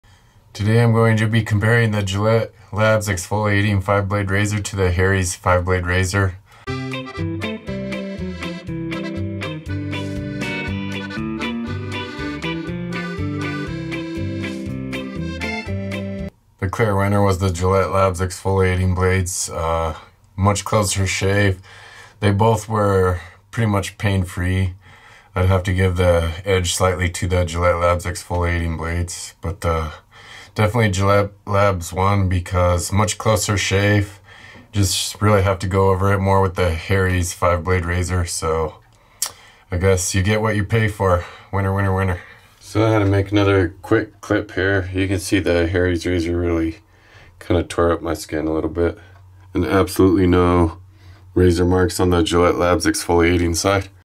Gillette Labs Exfoliating Blades Razor Vs Harry's 5 Blade Razor | Gillette vs Harry's Showdown
We compare Harry's five-blade razor to the Gillette LAbs Exfoliating five-blade razor...